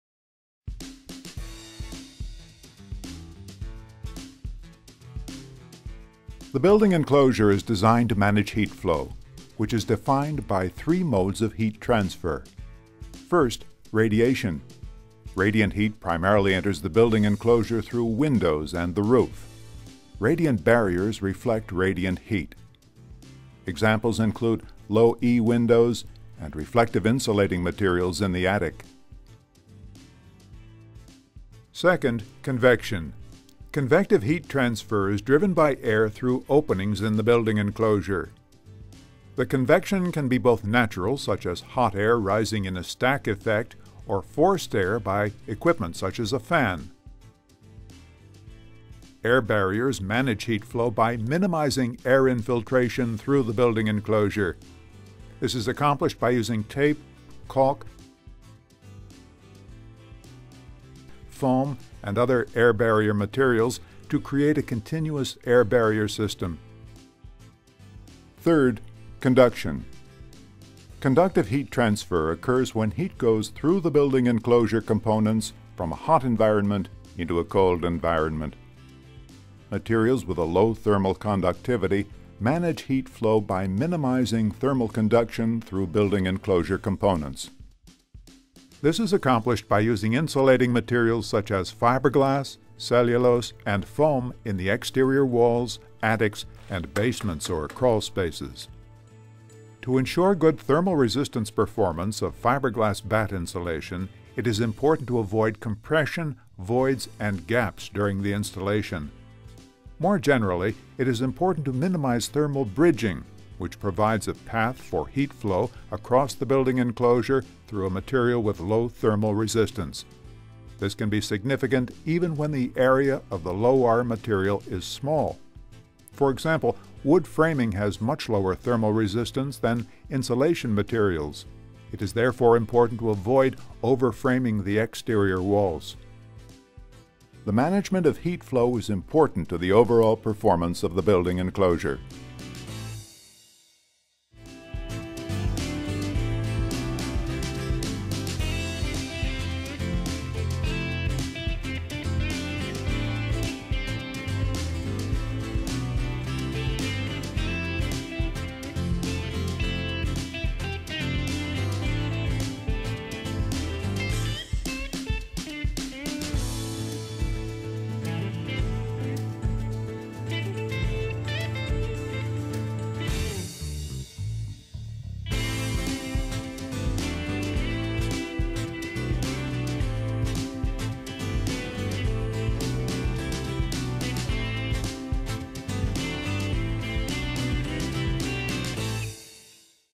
Moisture Management in Residential Construction - Building Enclosure: Managing Heat Flow
The building enclosure is designed to manage heat flow, which is defined by three modes of heat transfer: radiation, convection, and conduction. Topics in this video include radiant barriers, air barriers, thermal conductivity, thermal bridging, and insulation types.